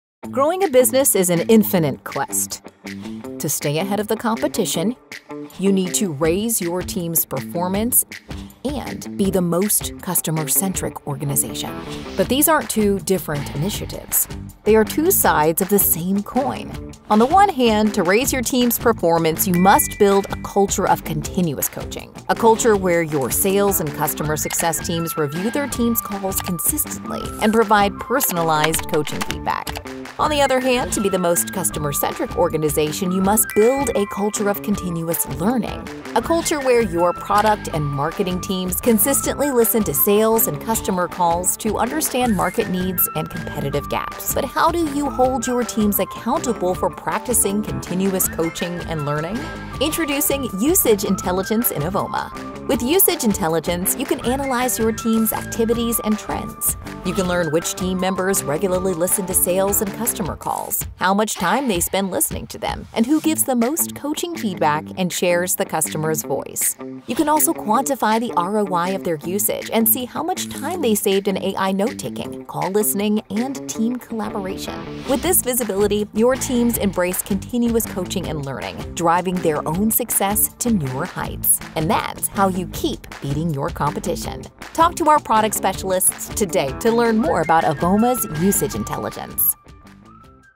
Track ROI from using Avoma and create a culture of continuous learning and coaching across your org
Keep Your Team on Target with Calls and Meetings

Discover how Avoma helps your team track call metrics, identify trends, and perform at their best. Understand the number of calls made, total time spent, and average duration. Identify trends across call types and meeting outcomes, such as demos, outbound efforts, or answered calls, to optimize team performance.

Build a Culture of Continuous Learning

Make continuous learning a KPI for all your teams! With Avoma's learning leaderboard, you can track who spends the most time listening to internal and customer conversations. This promotes cross-functional learning, helping marketers gain insights, product teams build valuable features, sales leaders coach effectively, and reps stay on top of their training.

Keep your Leadership Accountable for Coaching

Are your leaders reviewing team calls and providing feedback? Avoma's user-level activity trends hold leaders accountable for coaching. Track engagement by monitoring comments, shared meetings, and listened-to snippets to ensure that coaching drives impactful results.

See Your ROI Clearly

Never doubt the benefits of adopting Avoma. Gain clear insights into your team's productivity improvements with features like automated notes, one-click meeting sharing, and accelerated meeting playback. See how many hours your team saves and the tangible ROI from using Avoma.